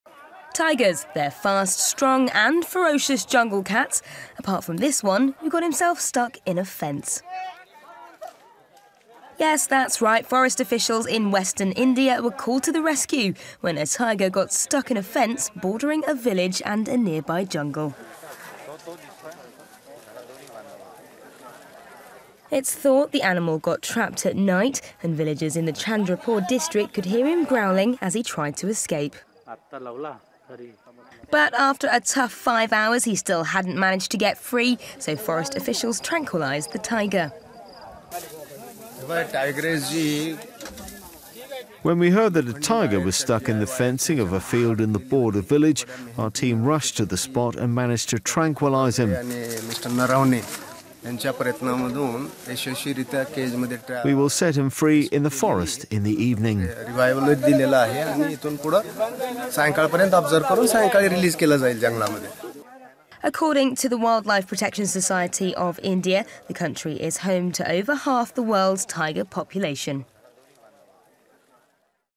Forest Officials Rescue Sub-Adult Tigress Stuck In A Fence
Forest officials freed tigress that was stuck in a fence bordering Borda Village. The tigress was stuck for five hours. Watch the video shot by villagers.